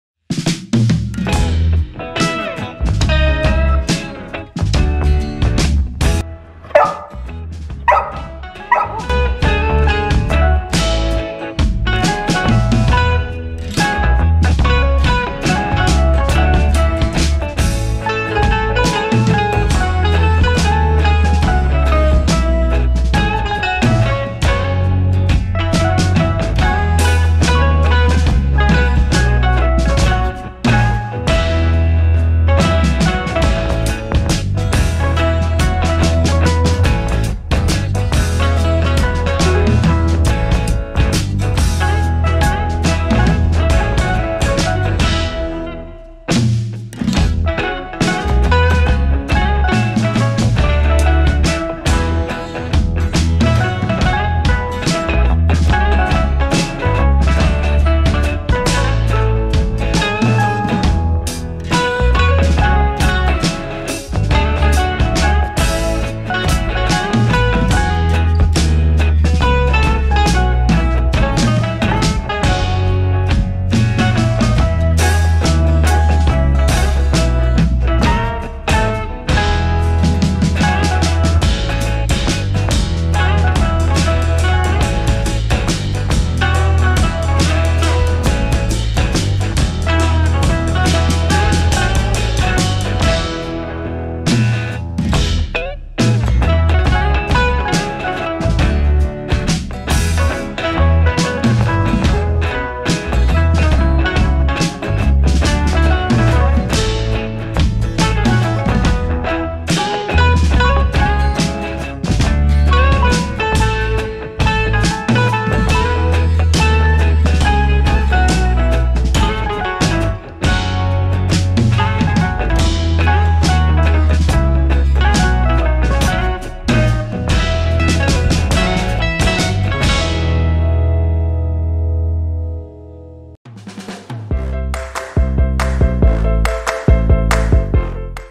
Our Golden Retriever Becomes a Painter
Watch Arpas attempt a dog painting challenge.

If you enjoyed the video make sure to enable push notifications!